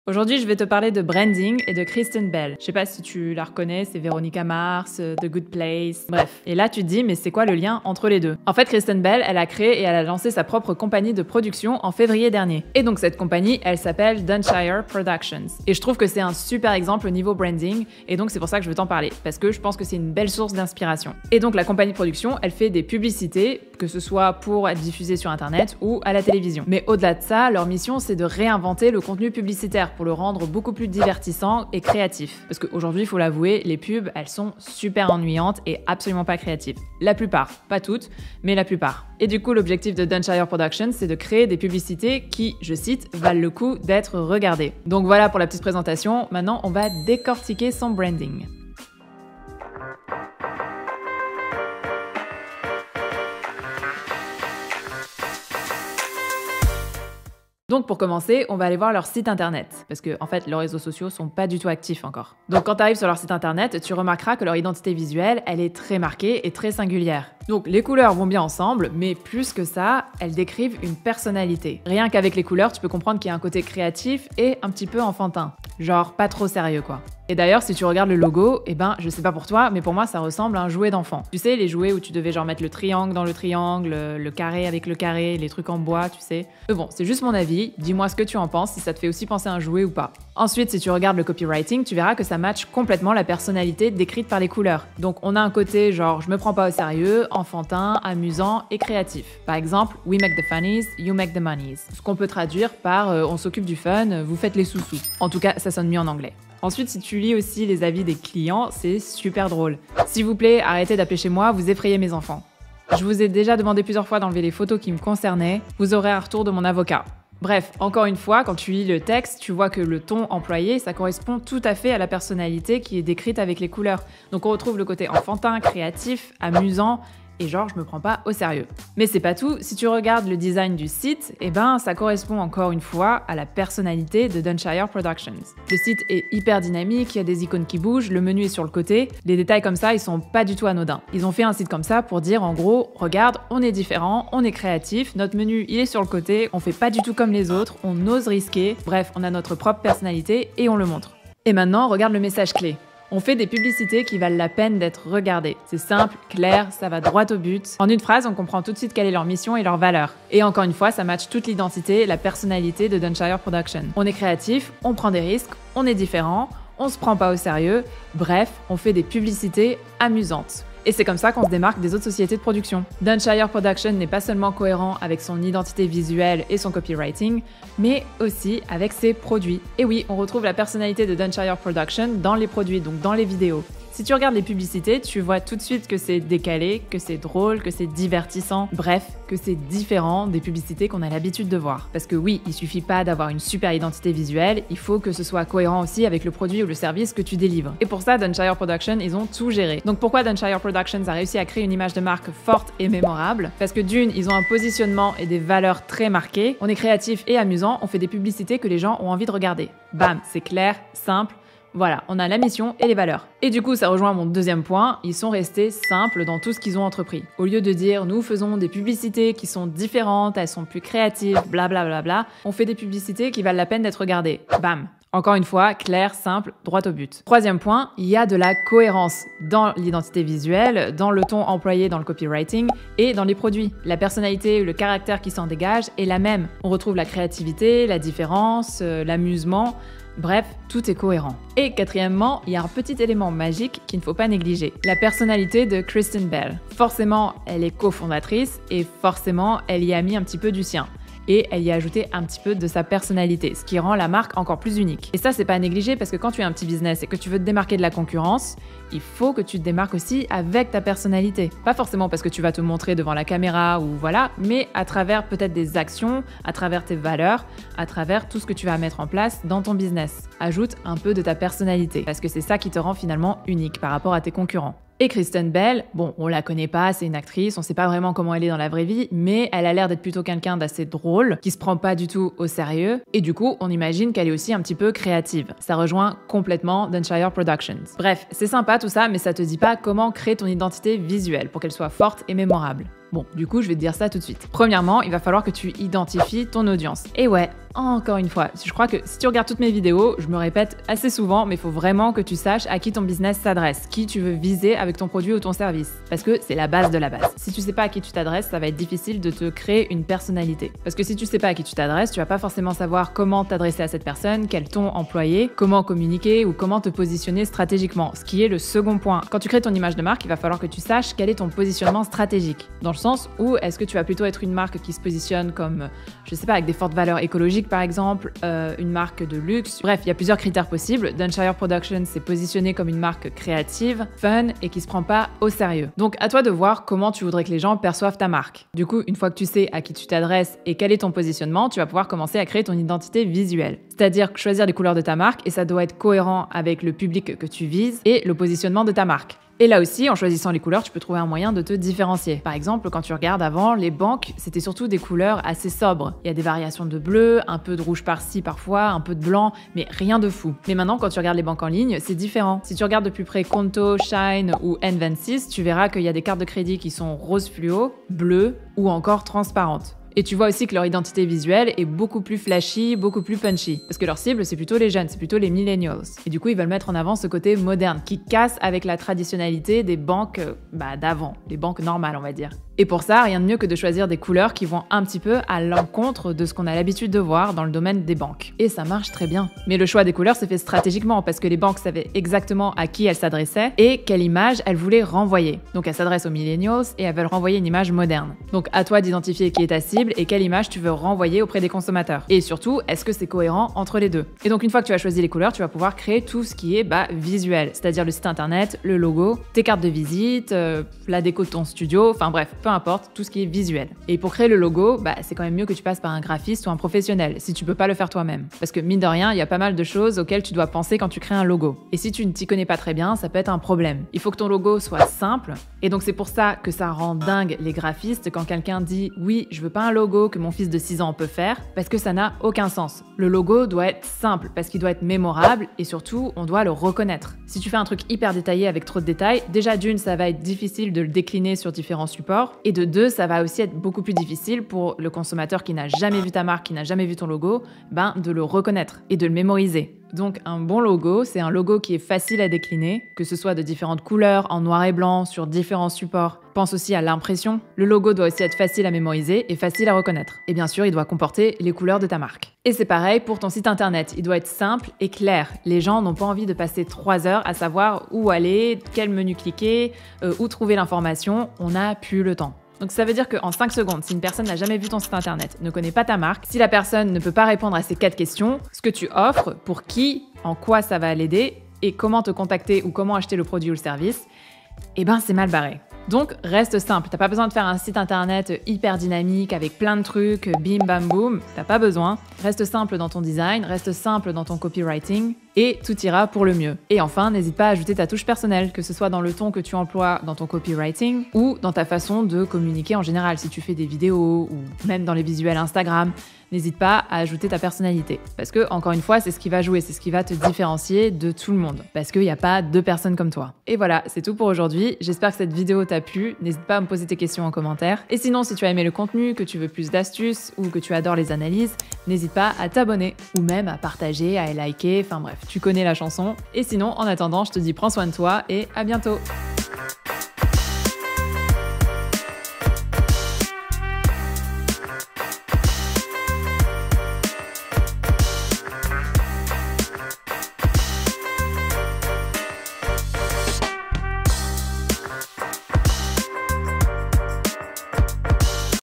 Comment créer une image de marque forte et mémorable selon Veronica Mars 👀
C'est quoi le rapport entre l'actrice Kristen Bell et l'image de marque (ou branding) ? 🧐

L'actrice a créer sa propre société de production en Février dernier, Dunshire Productions. Et je voulais t'en parler parce que c'est une belle leçon de branding 🤩 et une source d'inspiration si tu te demandes comment créer une image de marque forte et mémorable.

Pour rappel, l'image de marque comprend l'identité visuelle, les valeurs de la marque, sa personnalité, l'univers de la marque. L'image de marque doit être forte, mémorable mais surtout cohérente à travers toutes les actions que tu vas entreprendre avec ton business ! 👌🏻

Vidéos d'illustrations :

Pexels.com
Giphy.com